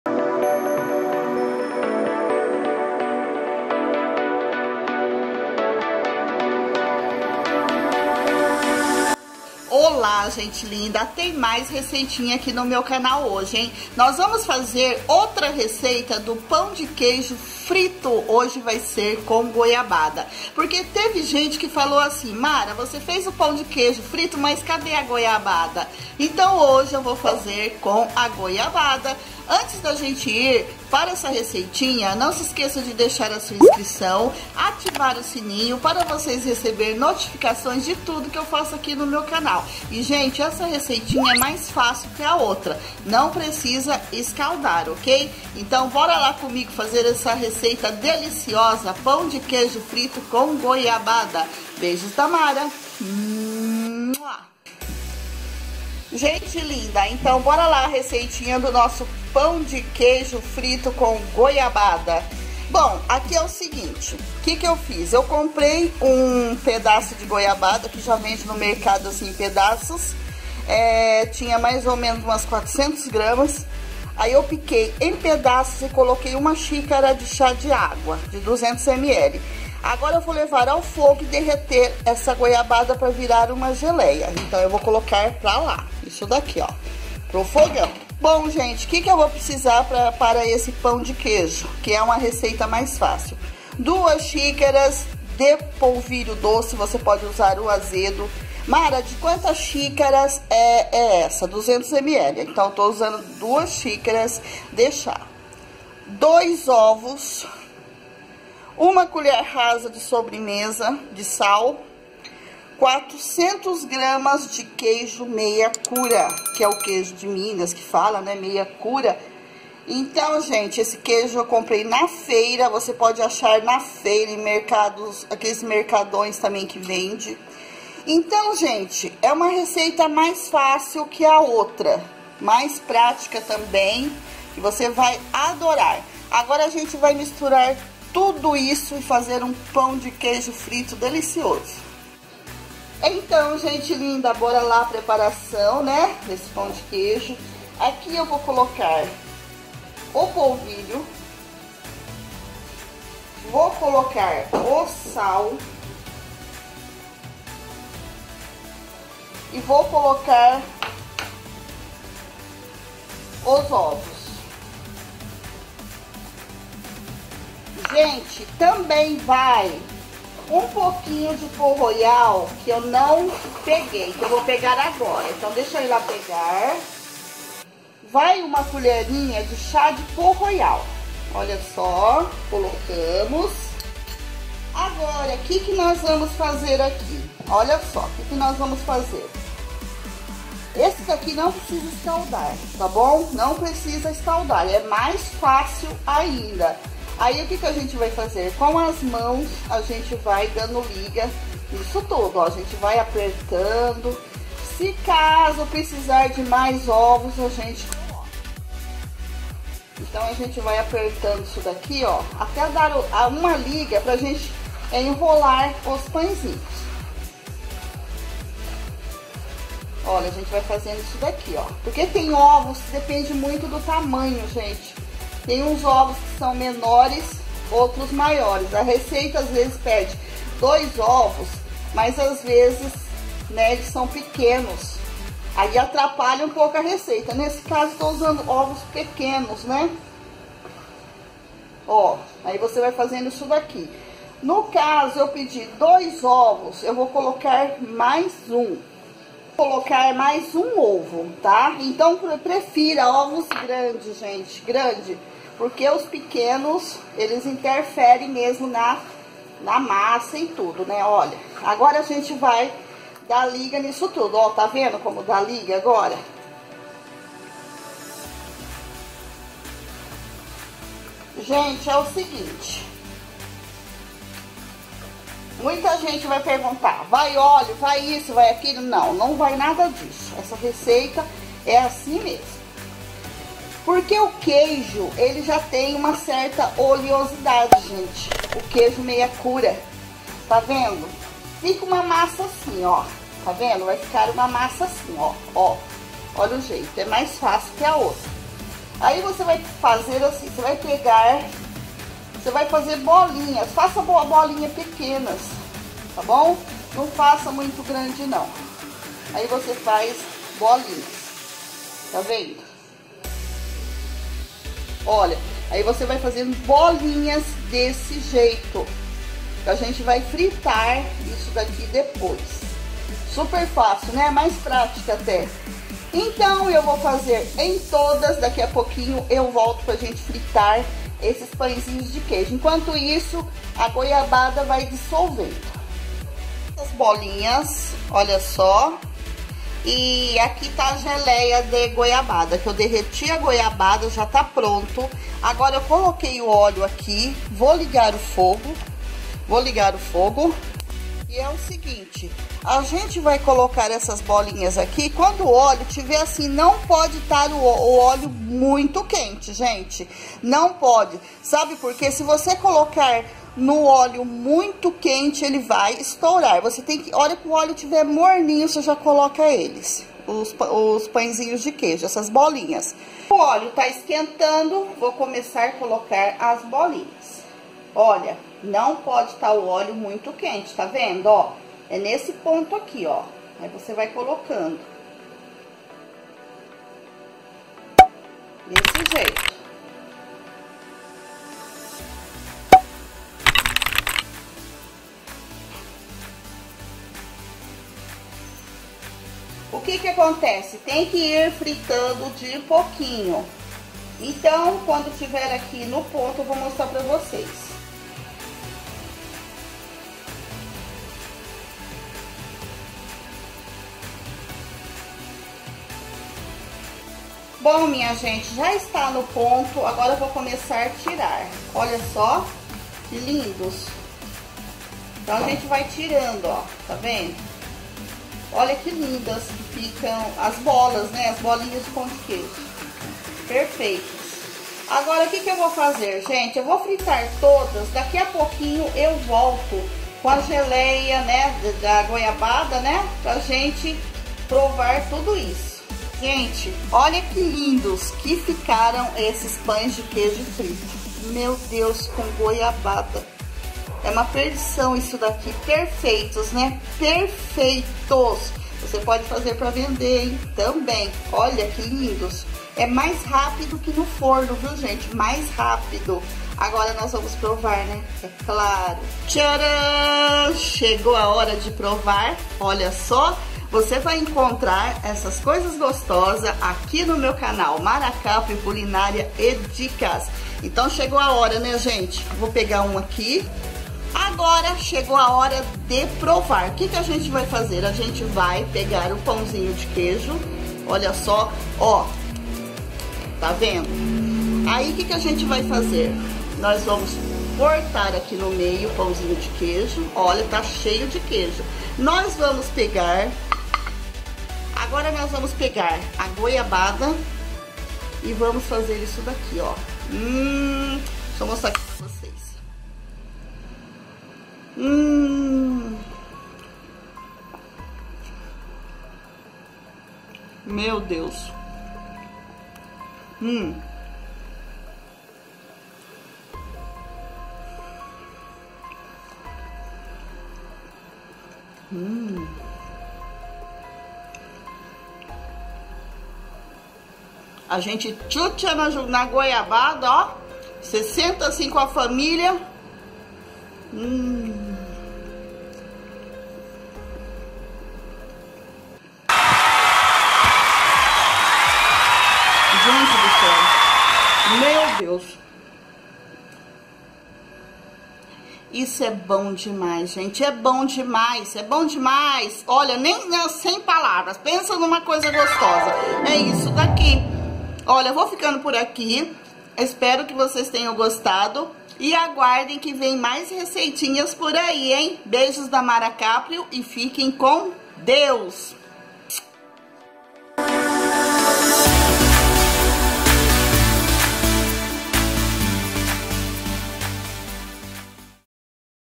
PÃO DE QUEIJO FRITO COM GOIABADA RECEITA MASTERCHEF MUITO FÁCIL E RAPIDA POR MARA CAPRIO
BORA LA A RECEITINHA

400 GRAMAS DE QUEIJO MEIA CURA OPÇÃO PODE USAR MUSSARELA RALADA
3 OVOS GRANDES
2 XÍCARAS DE  CHÁ POLVILHO DOCE PODE SER O AZEDO
1 COLHER DE CHÁ DE PÓ ROYAL